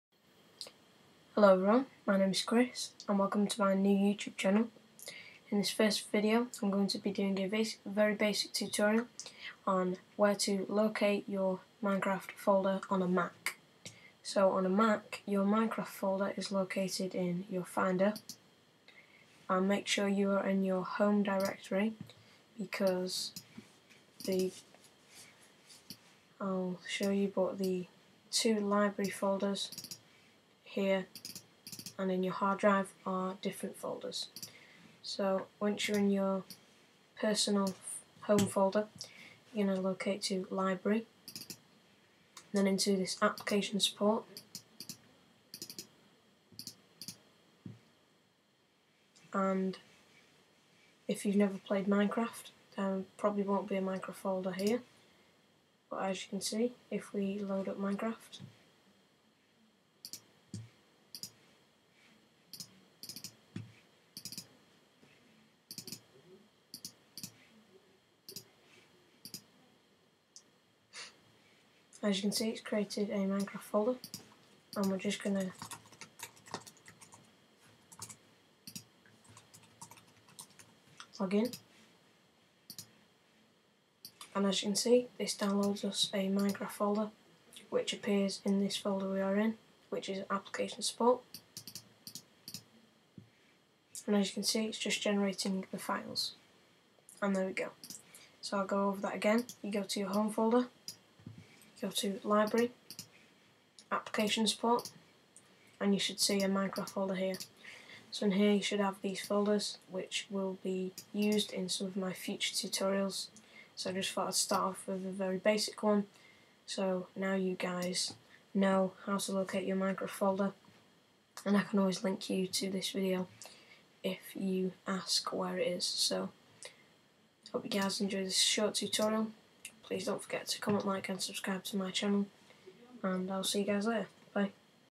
How to locate your Minecraft Folder on a Mac (1080p)
Very simple tutorial on how to locate your Minecraft Folder on a Mac.

The root is:
Home Folder  - Library - Application Support - minecraft